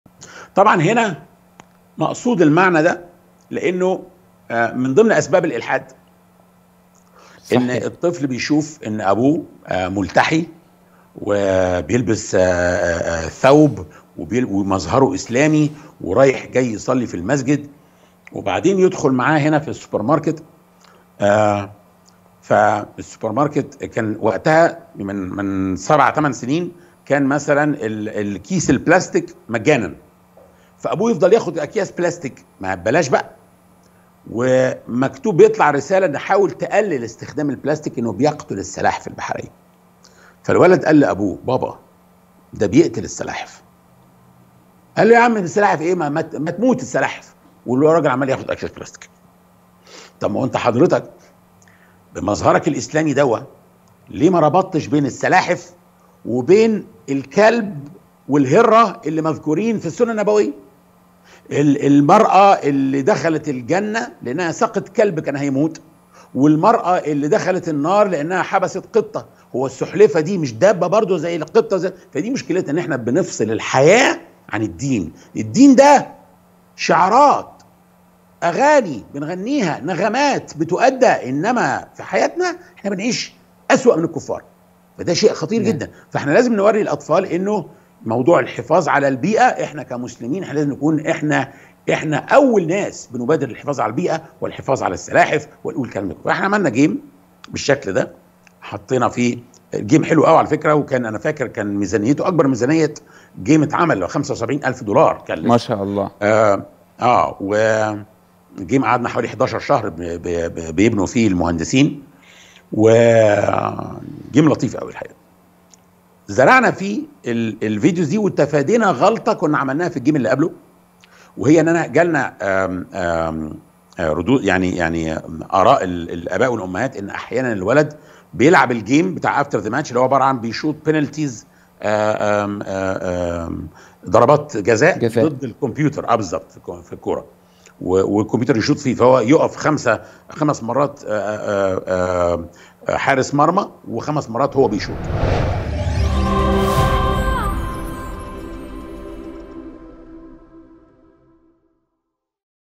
فاضل سليمان يروي واقعة كيس البلاستيك والسلاحف البحرية
Nilesat (النايل سات): 11603 أفقي